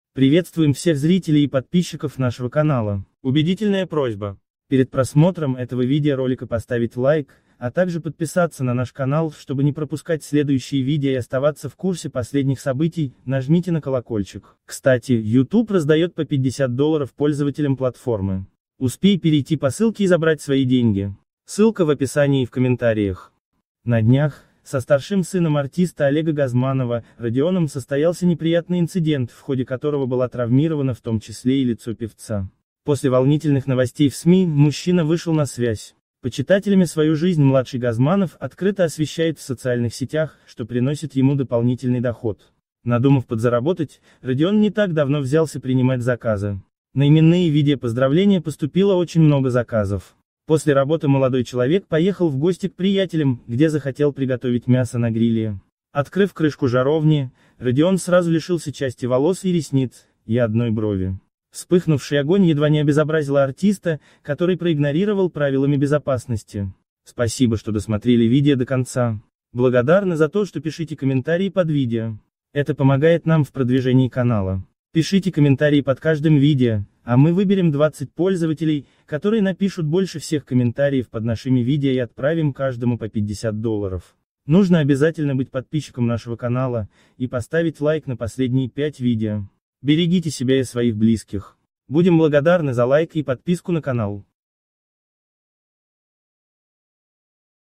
Гopeл зaживo. Певец Родион Газманов
Смотрите Гopeл зaживo. Певец Родион Газманов

Известная российская и советская  певица Алла Пугачева, в связи с особой обстановкой в стране, покинула Россию и переехала в Израиль вместе с детьми на постоянное место жительство
Для этого Алла Пугачева воспользовалась услугами своего двойника- Натальи Буйницкой.
Ее муж-пародист -Максим Галкин тоже покинул Россию и уехал в Турцию.
Алла Пугачева и Максим Галкин в ближайшее время не собираются возвращаться в Россию.

 @НОВОСТИ ОНЛАЙН